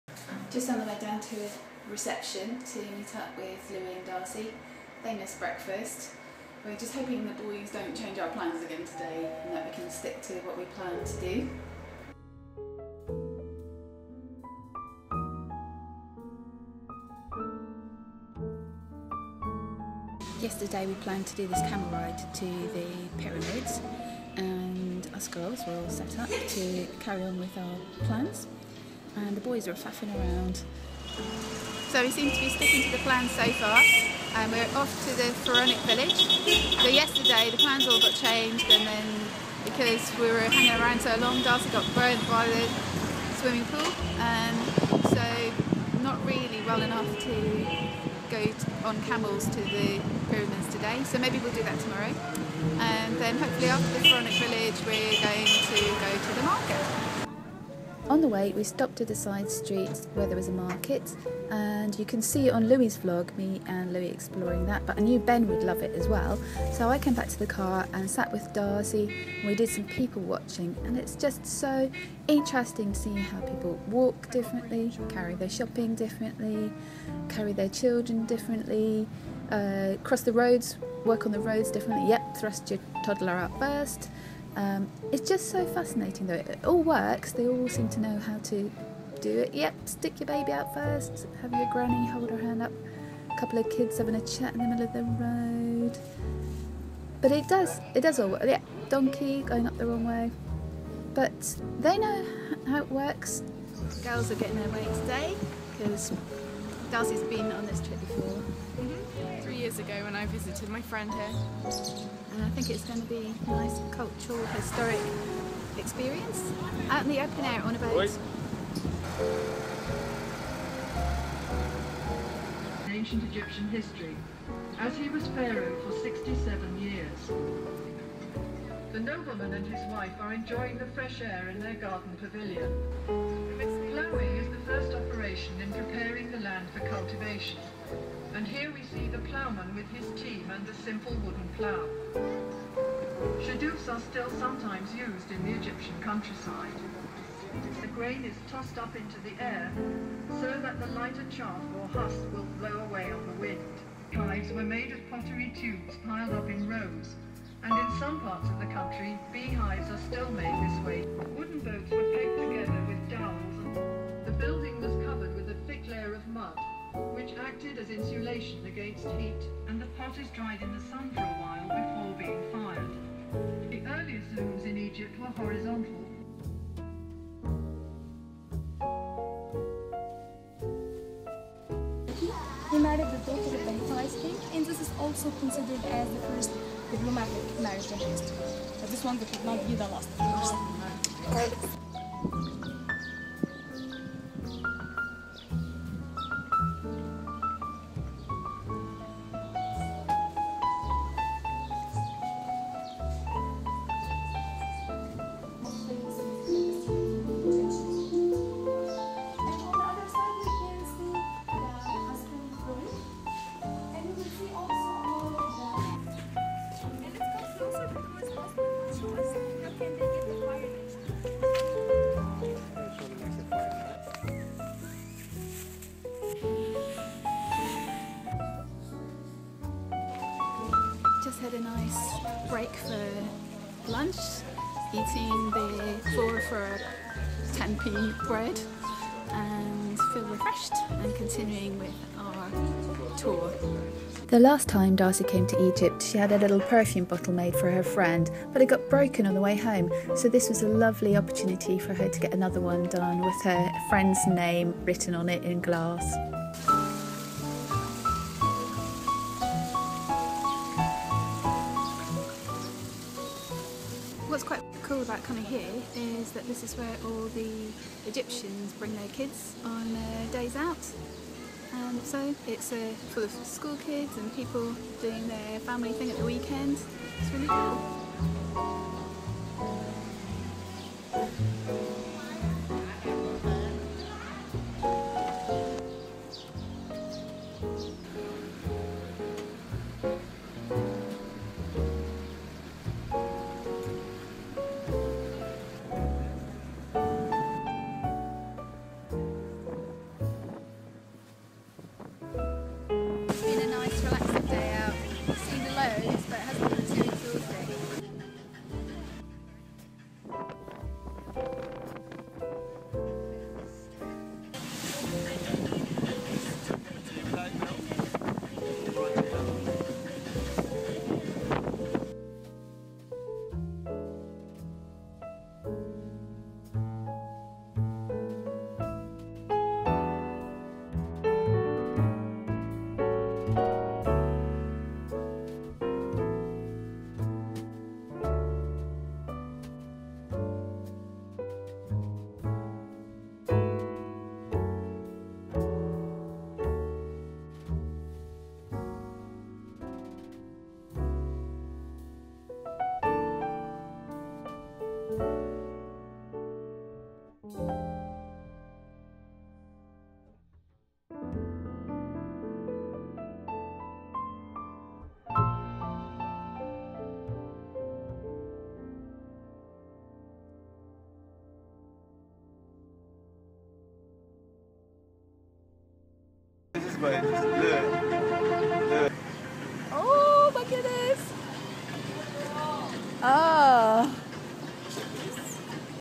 Cole Family Adventure Day 4 | Near Death on the Nile
Discovering Egypt.  Relaxed day out at the Pharaonic Village, Cairo (with a scary bit at the end).  This is the less professional vlog of our family holiday but a bit more behind the scenes.  Here's Louis Version